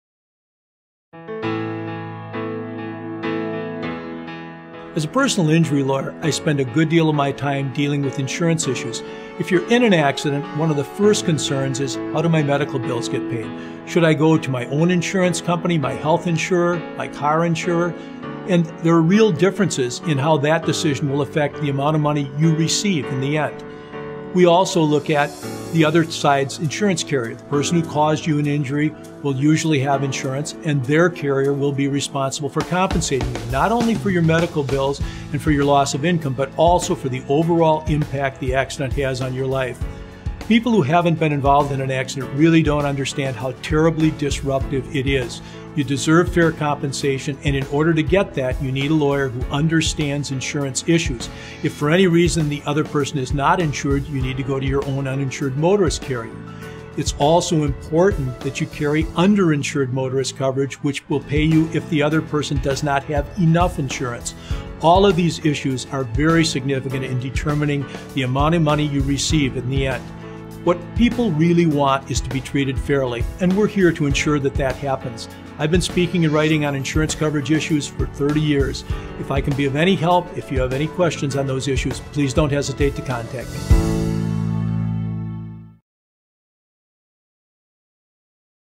Personal Injury Basics: What Role Does Insurance Play When You're Injured?
Madison Wisconsin personal injury attorney Mike Riley explains the role insurance plays when you're injured and why you need an accident attorney who understands insurance issues. Learn more about Axley's personal injury services